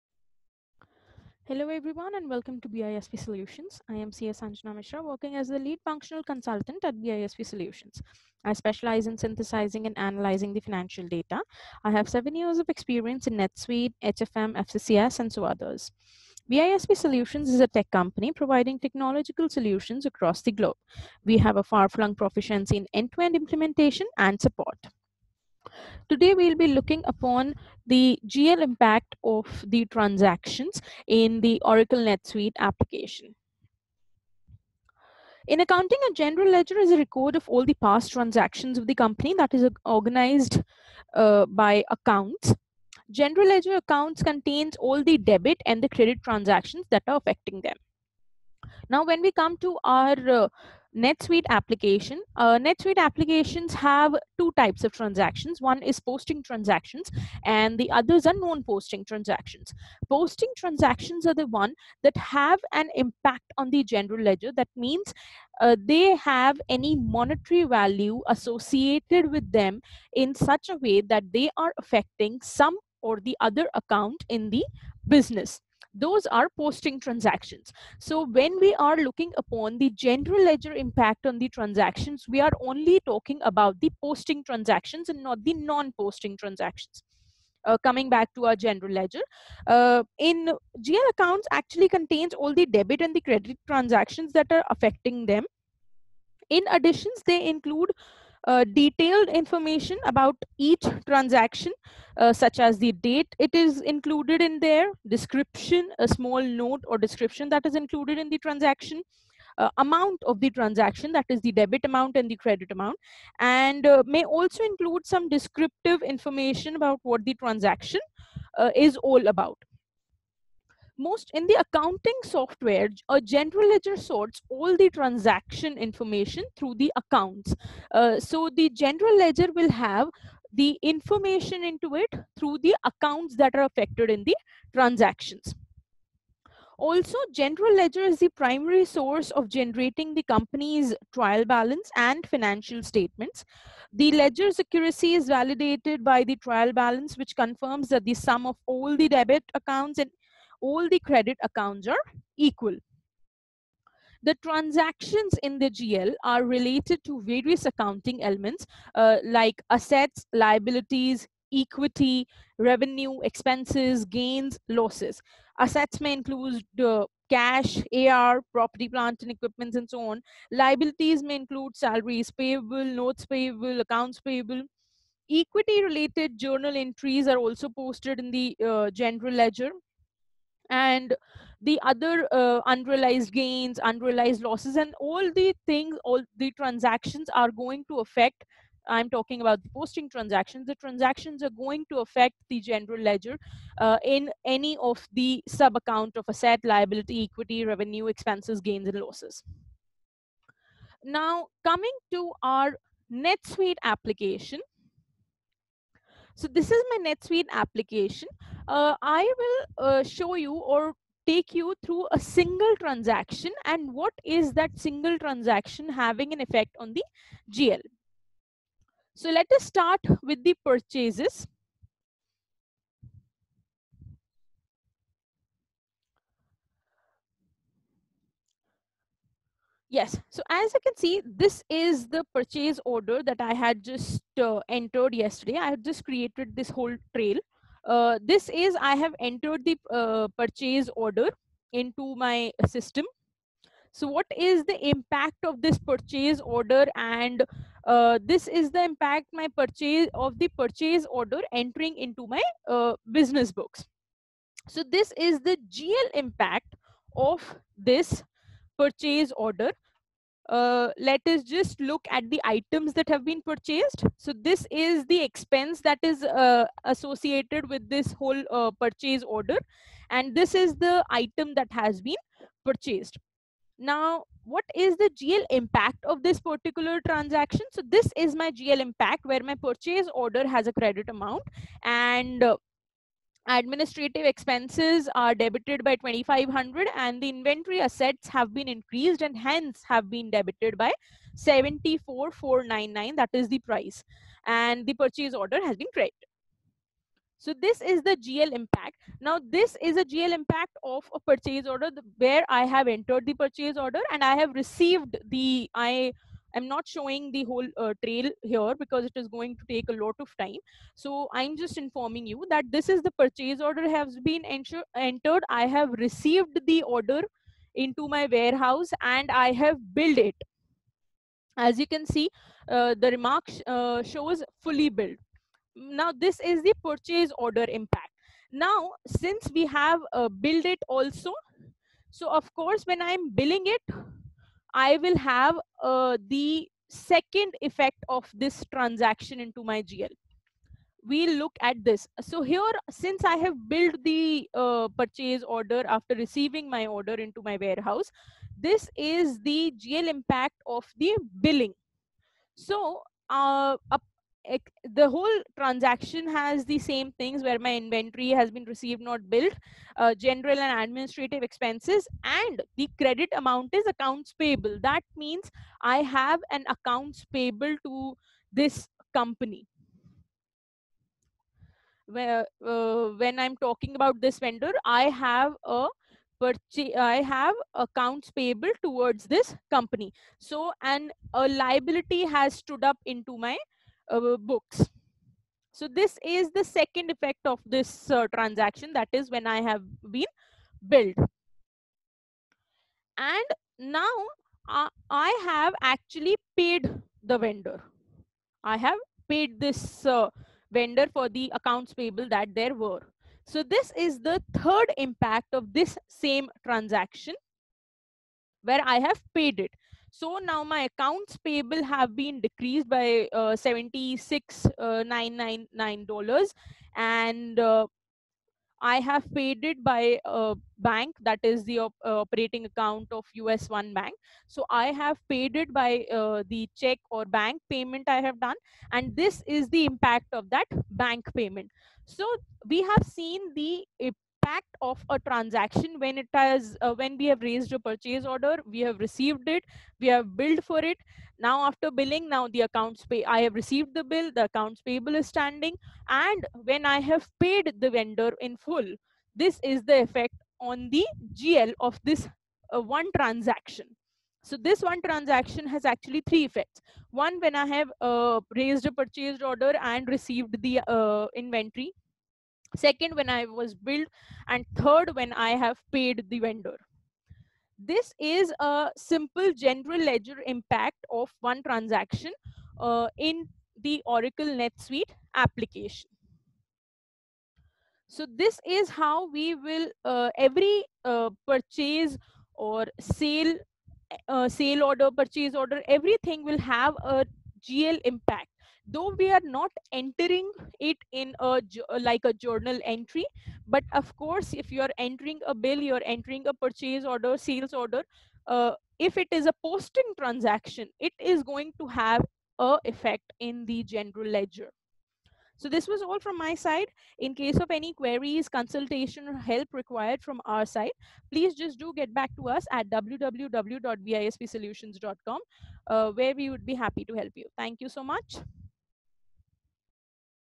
Oracle NetSuite GL Impact | Oracle NetSuite General Ledger | NetSuite Training
In accounting, a General Ledger (GL)is a record of all past transactions of a company, organized by accounts. General Ledger (GL) accounts contain all debit and credit transactions affecting them. This video tutorial shows how Oracle NetSuite GL Impact on any transaction. those transactions that have GL impact are called posting transactions. This is a combination of concepts and Hands on session. We will explain Oracle NetSuite General Ledger.